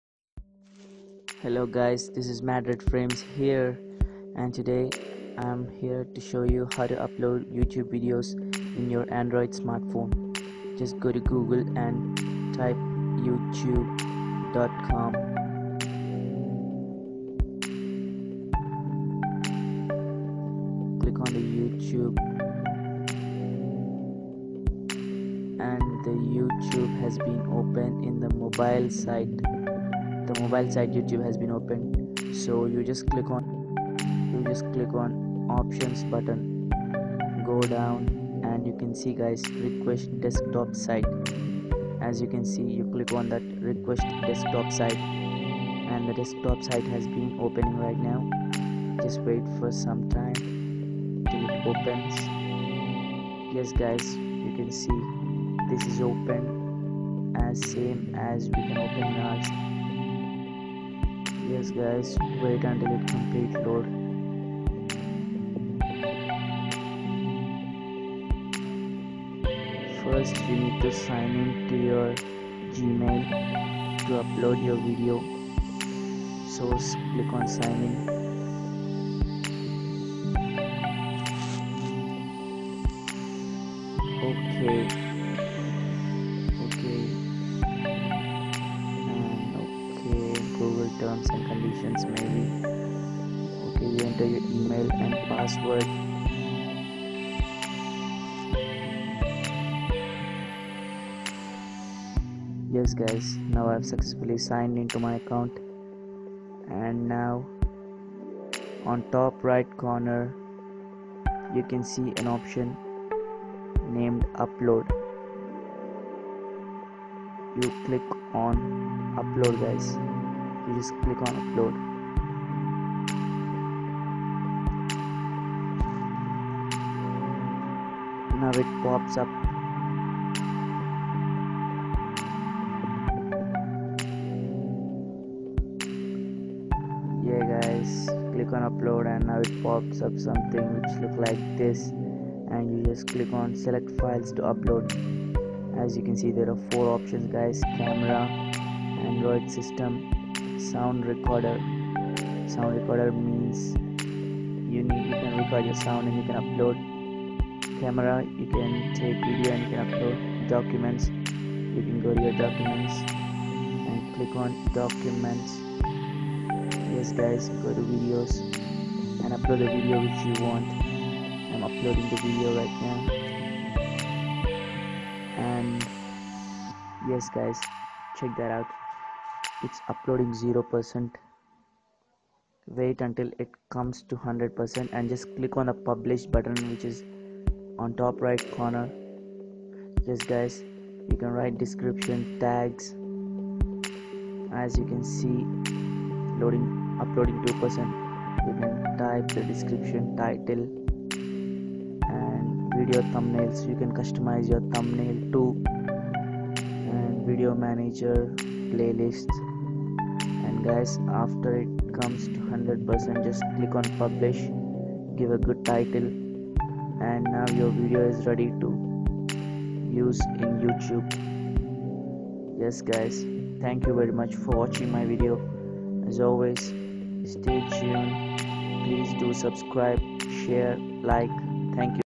HOW TO UPLOAD VIDEO'S IN YOUTUBE LIKE PRO IN ANDROID PHONE SIMILAR TO PC MADRED FRAMES MADHAN.
Must watch guys  uploading video in YouTube in a android phone which is pretty much similar to how you upload in PC .no need of PC if you see this vedio All you can handle with a smartphone you can also customize your thumbnail .2016